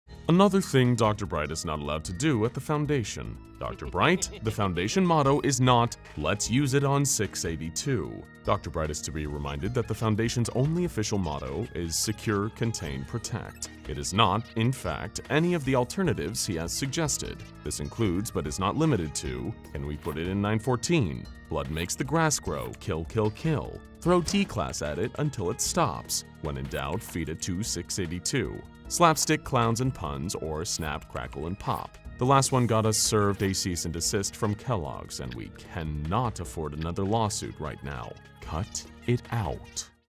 Dr. Bright! The Foundation Motto is Not "Let's Use it on 682!"
Largely due to the fact that he is begrudgingly stuck being immortal, Dr. Bright is known to be a jokester from time to time at the SCP Foundation.
There is a list of things Dr Bright is not allowed to do at the foundation.
This video explains one of those things.

Based on: The Things Dr Bright Is Not Allowed To Do At The Foundation by AdminBright